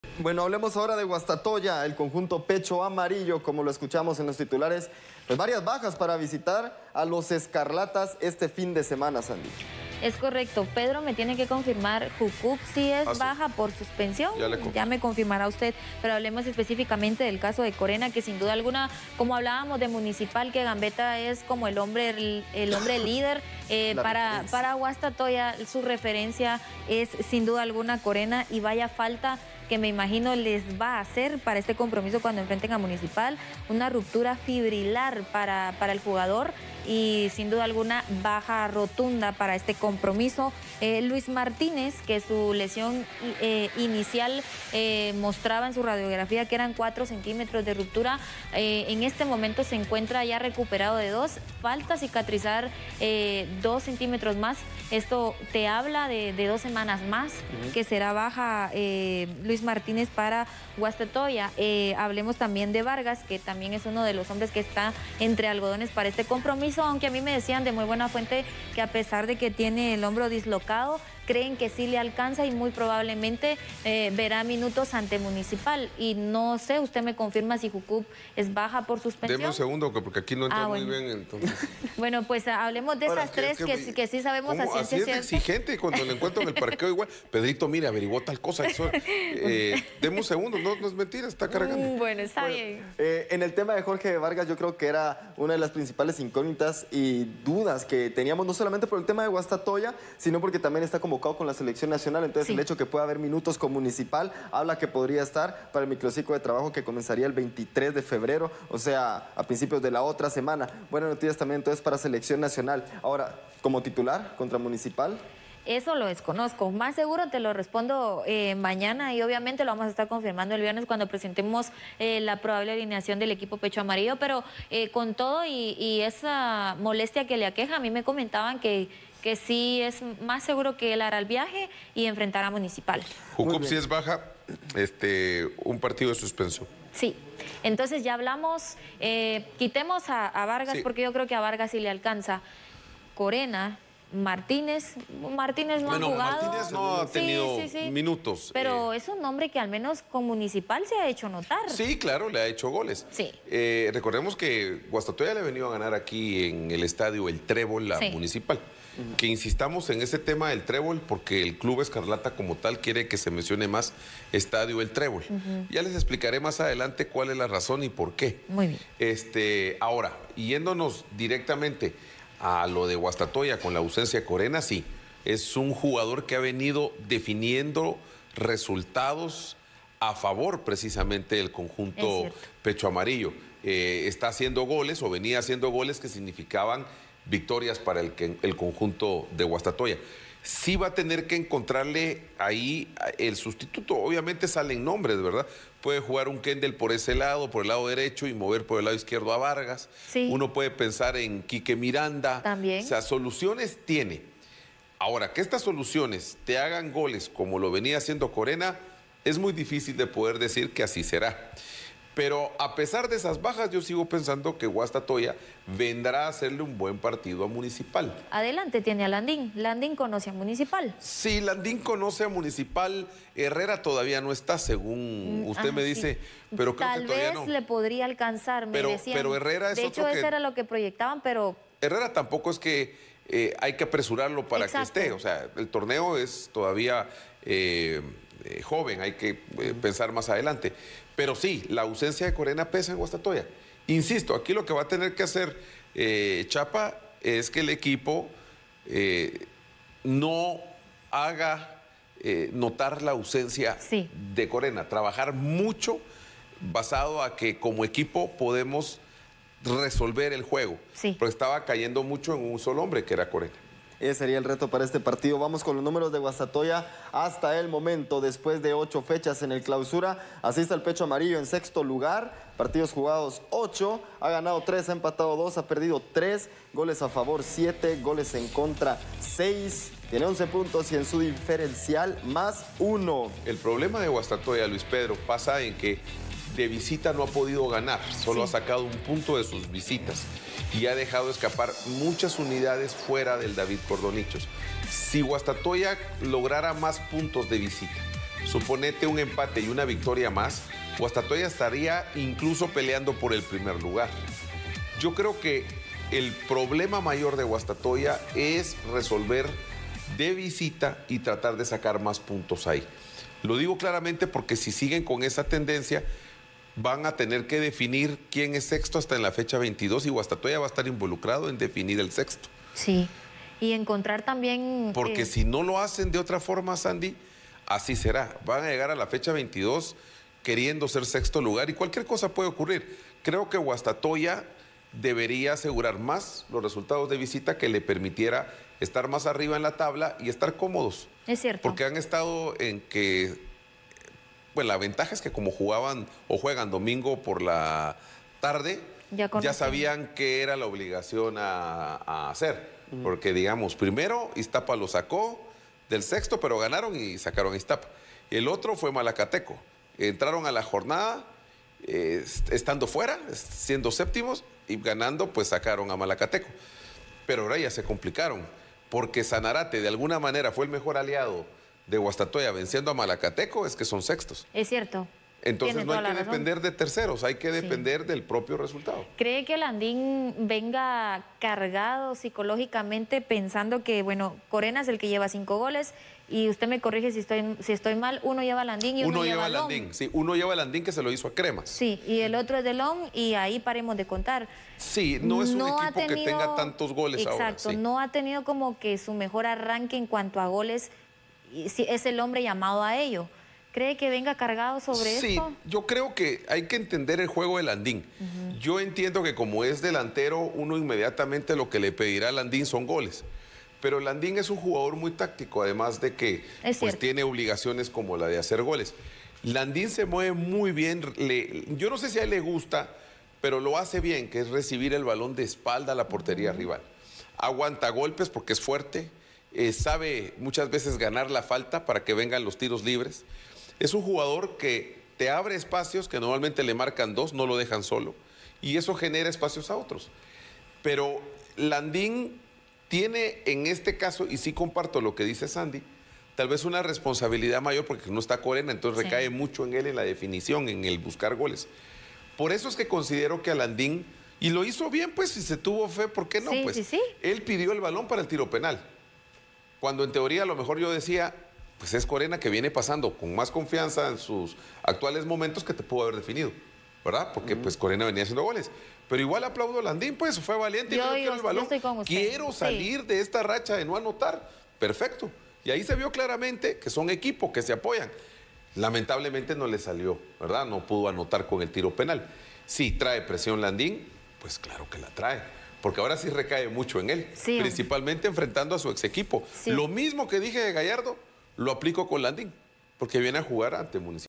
Torneo Clausura: Guastatoya llega con varias bajas para duelo ante Municipal
A propósito del cotejo entre Municipal y Guastatoya por la jornada 9 del Torneo Clausura del fútbol nacional, el conjunto 'pechoamarillo' deberá sortear varias bajas ante su par 'escarlata'.